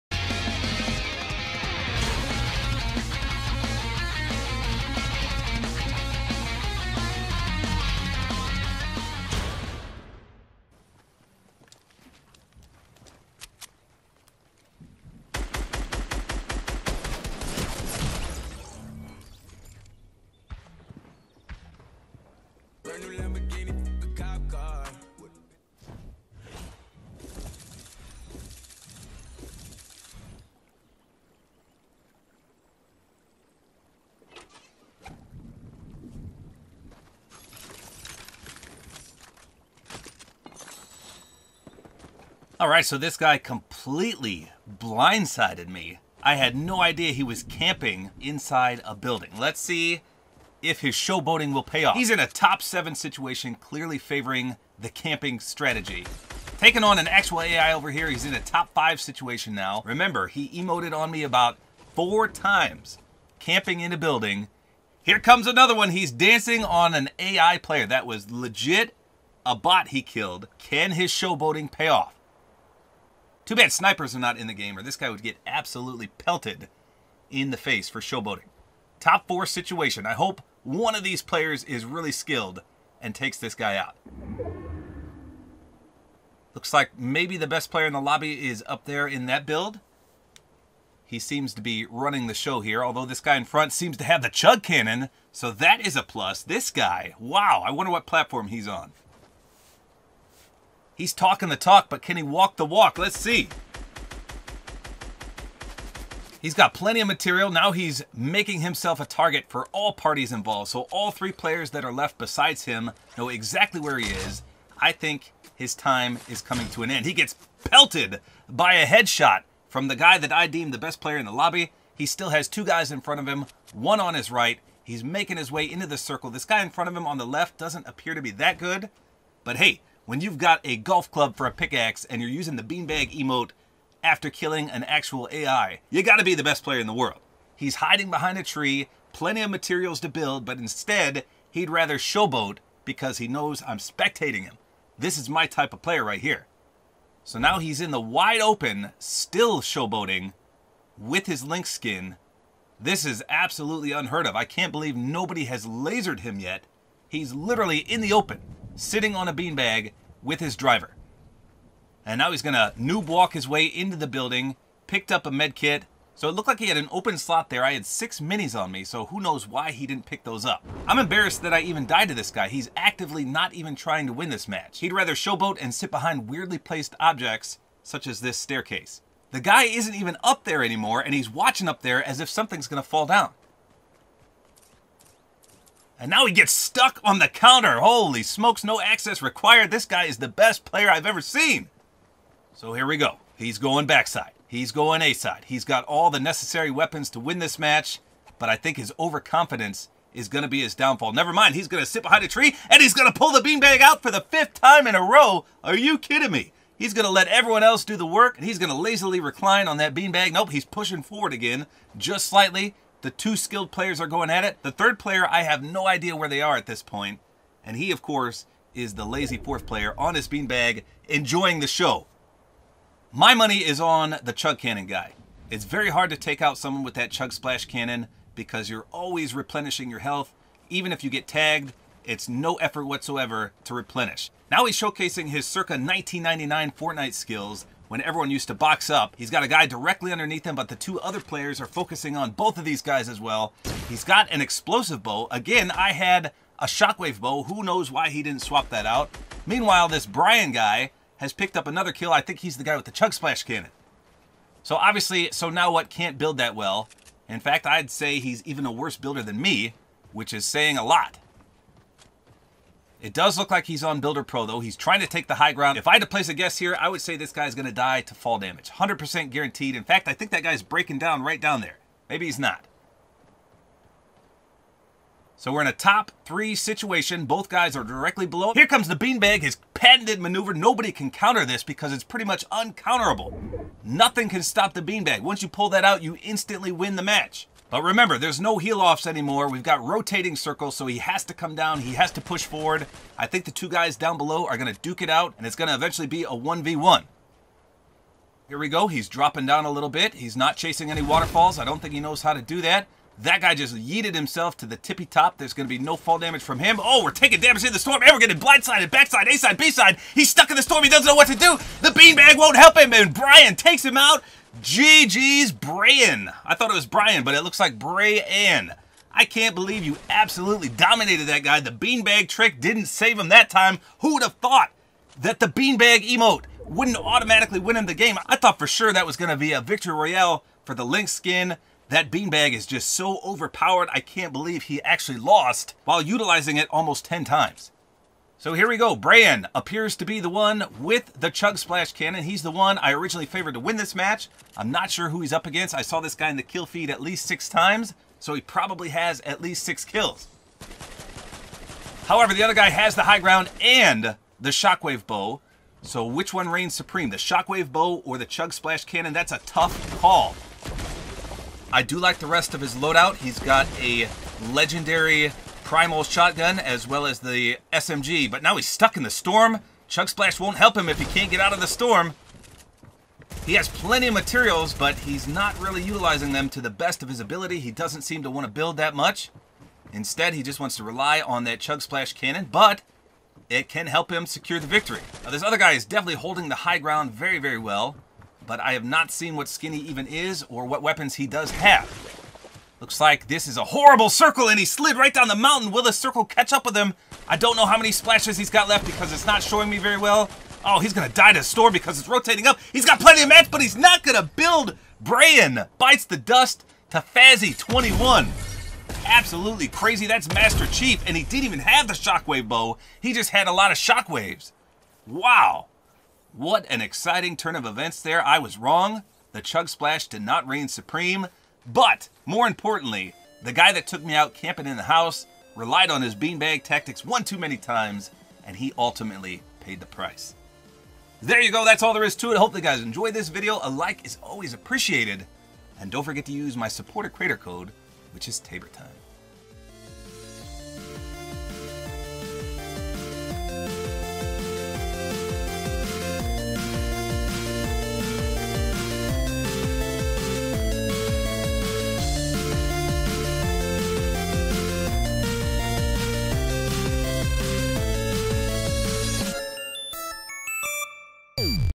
This Guy Tried To Win A Match Using Only The BEAN BAG Emote!
After blindsiding me while camping in a house, he proceeds to do the impossible:  win a match using only the Bean Bag emote.